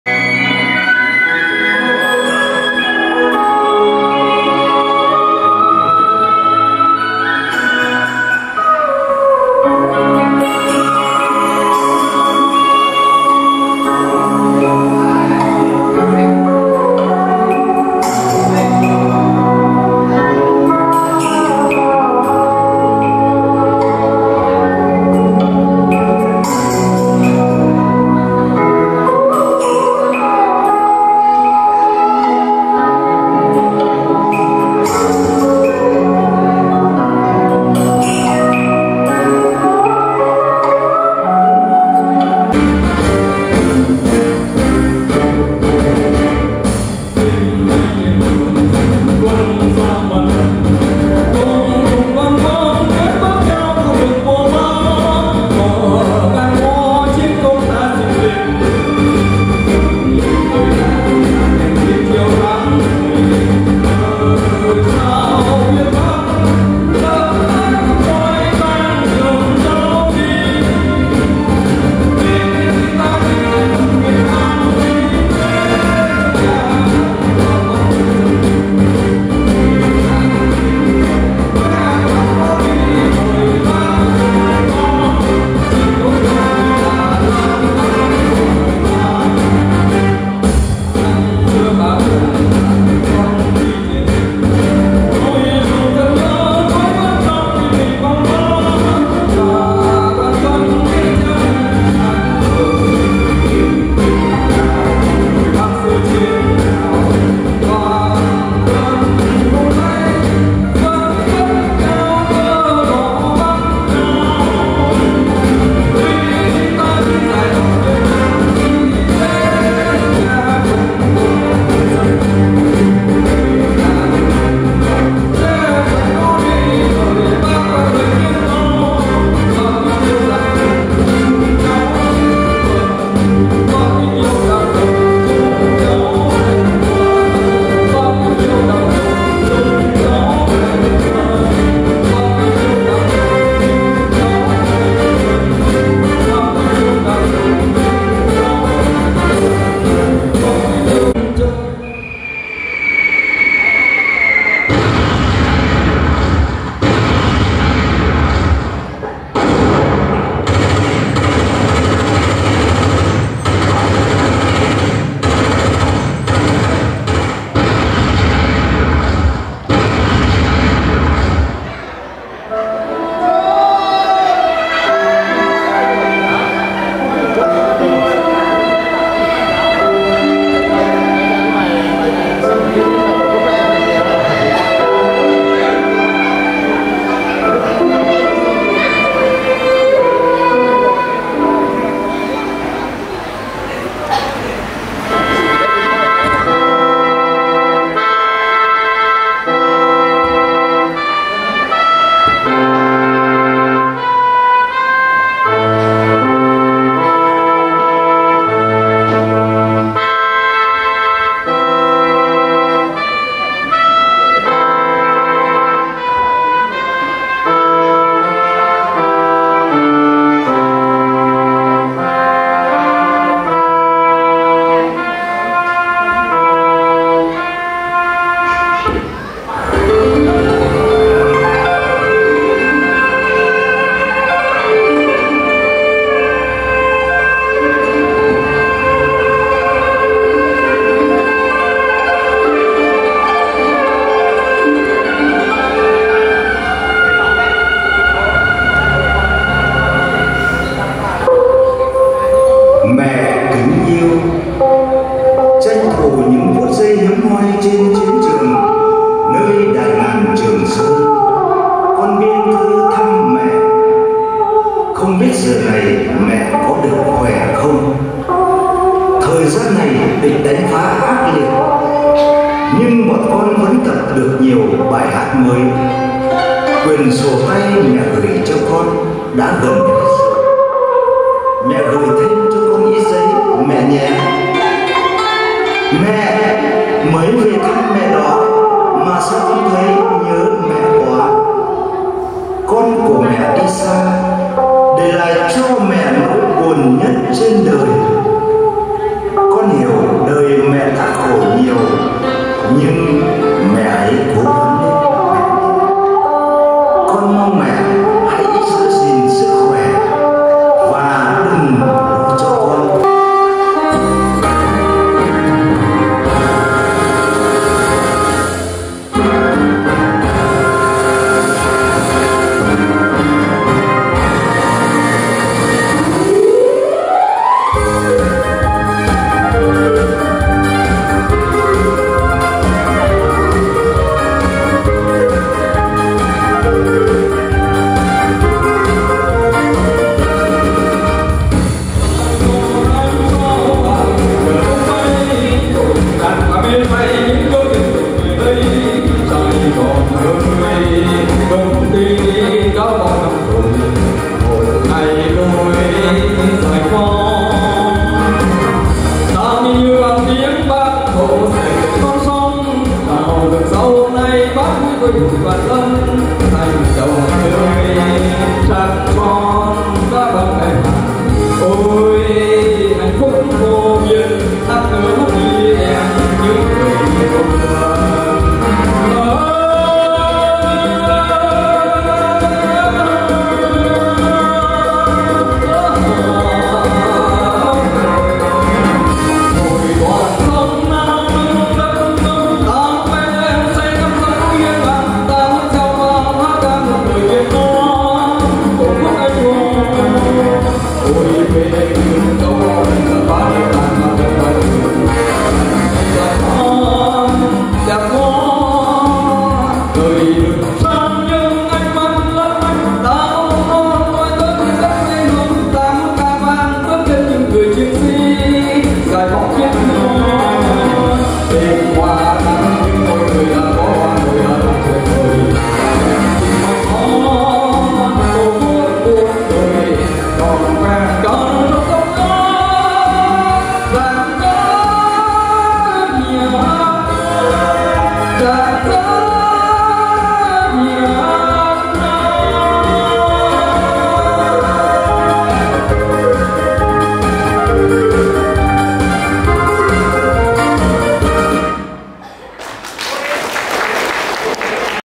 Tiết mục lấy đi nhiều nước mắt của khán giả |Thôn Tiên Hòa 2 - Hà Lĩnh
Ngày hội toàn dân bảo vệ an ninh tổ quốc.